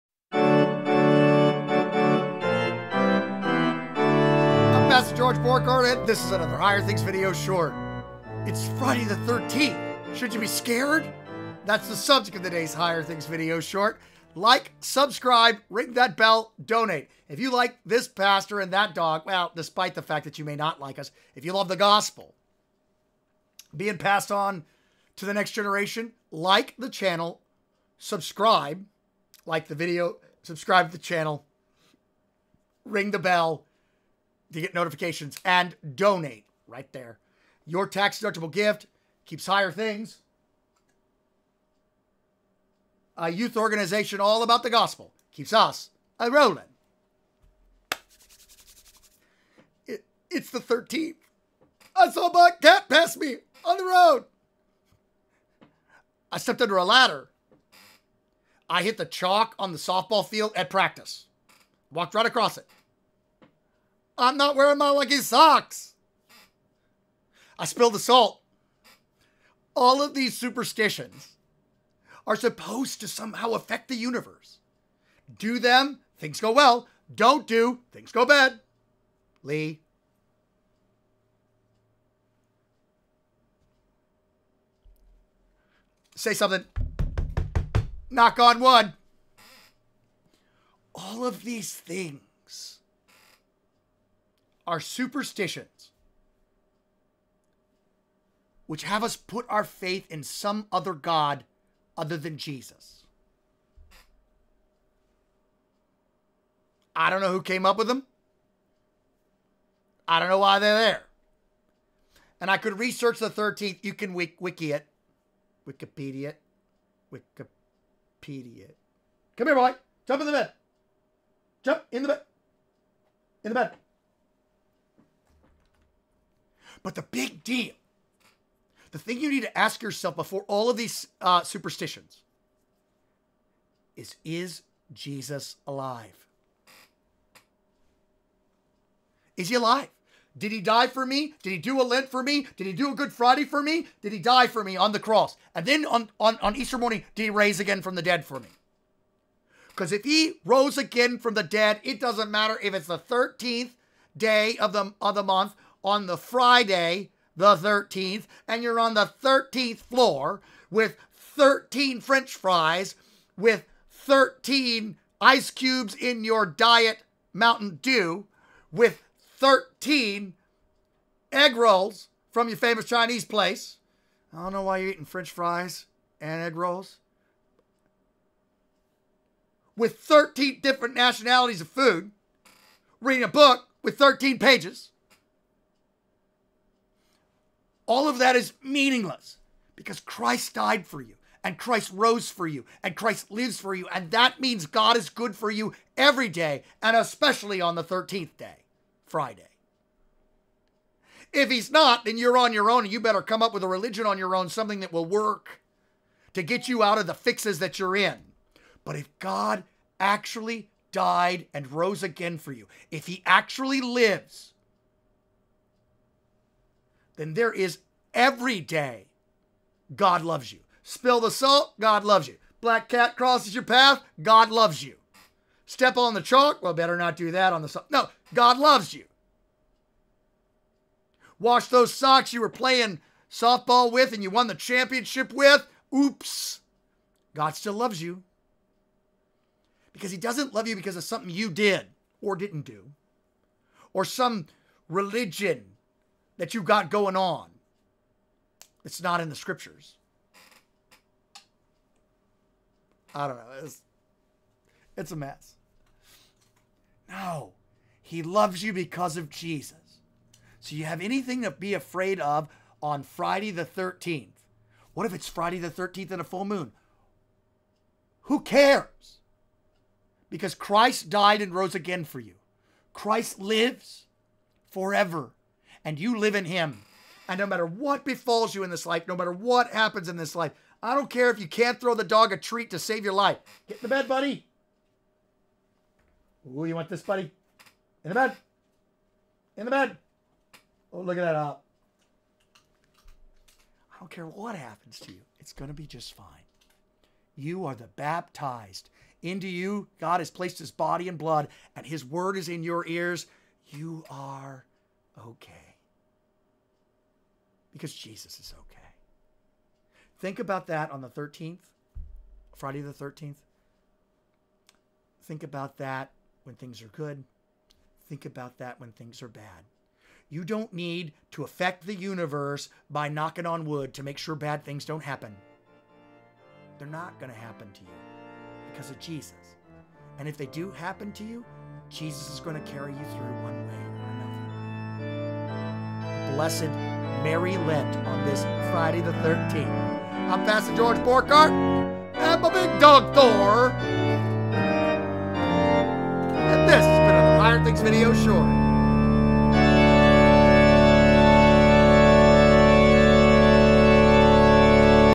0313 Triskaidekaphobia - Should You Be Scared? - A Higher Things® Video Short
The thing you need to ask when about superstitions is: Is Jesus alive? Did He die for you? And did He rise again? Because Christ died and rose from the dead, that means that God is good for you every day. No matter what happens in your life, it's going to work out just fine. You are the baptized. Jesus is going to carry you through, one way or another.